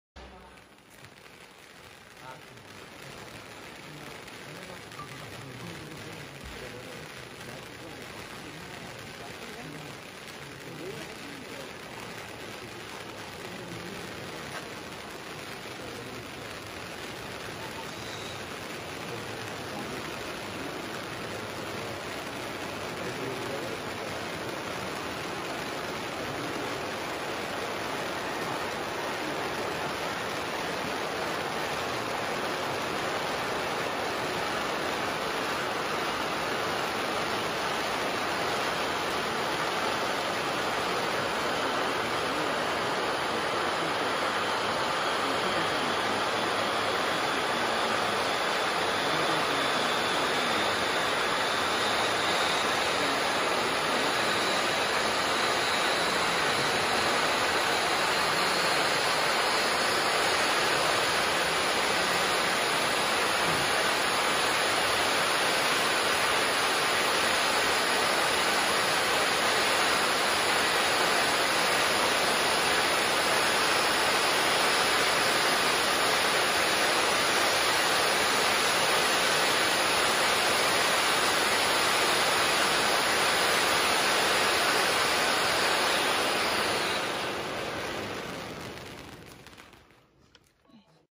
Celtic Fireworks Tears of an Angel Fountain x 3
Celtic Fireworks Woooooshhh Fountain x 3. This stock footage shows a trio of fountains in an arrangement to show off what can be done with the two variants of this fountain - one goes from gold to silver whilst the other silver to gold.